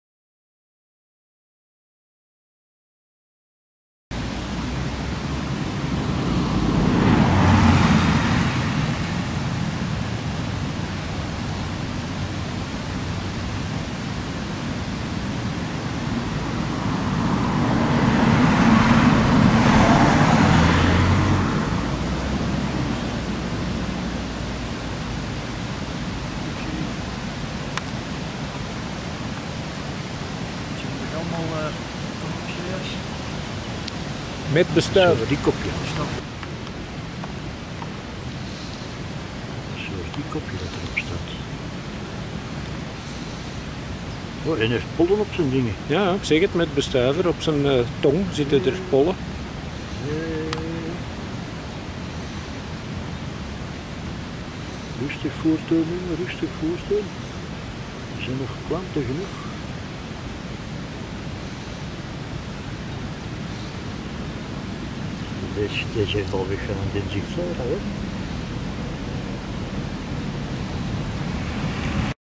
Gymnadenia conopsea heeft bezoek
Gymnadenia conopsea heeft bezoek van een Kompassla-uil / Hecatera dysodea. Opnames gemaakt langs de kant van de weg in de Franse Alpen.